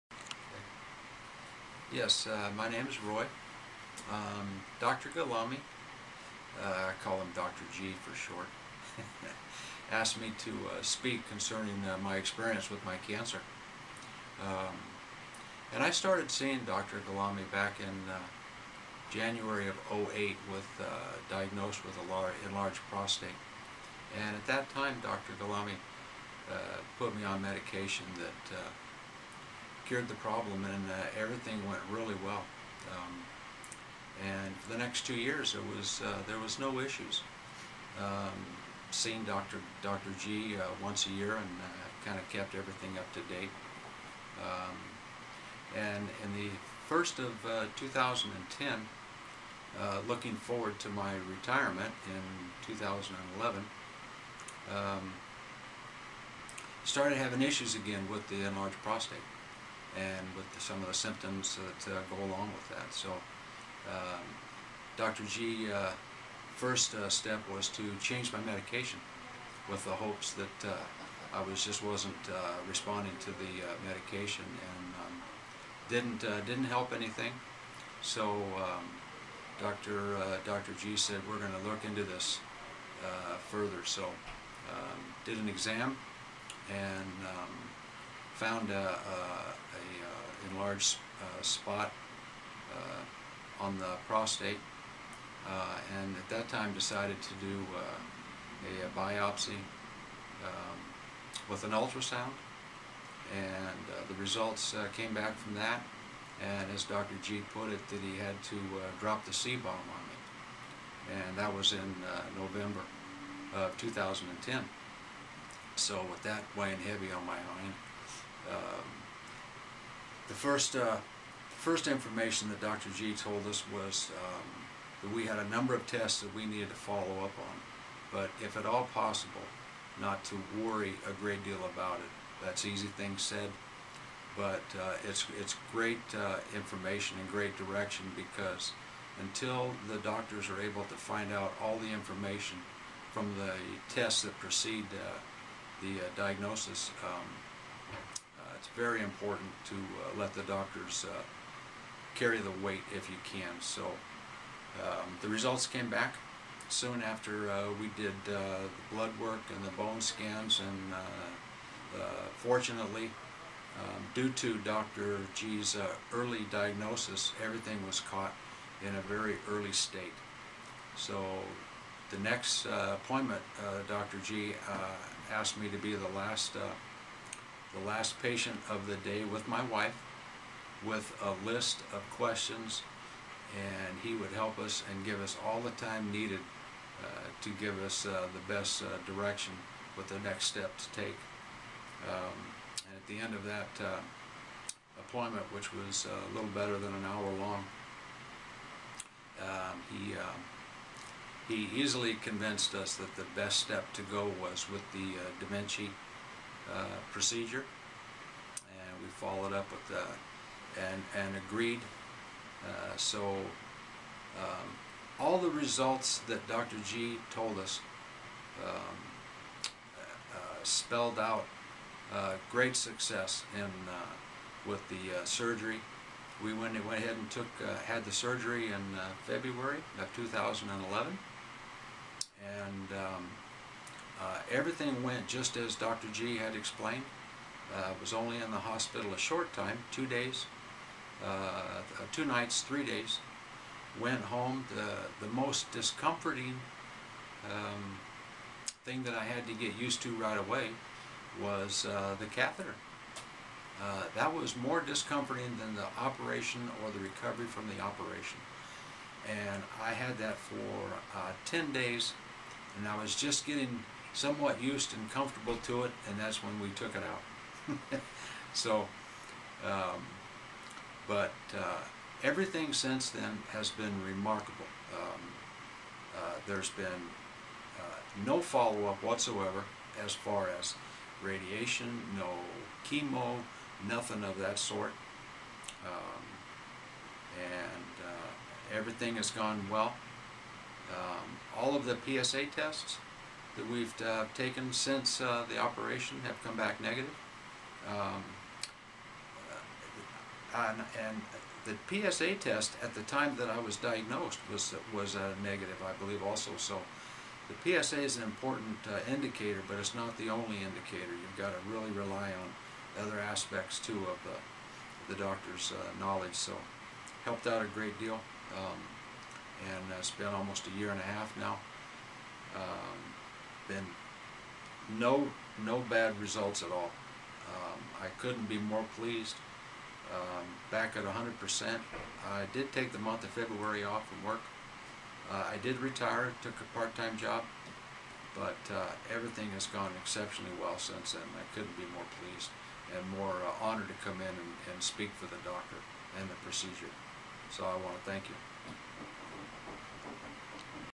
Patient Testimonial #5
One of Dr. Gholami's patients offers a testimonial of his experience with his prostate cancer treatment.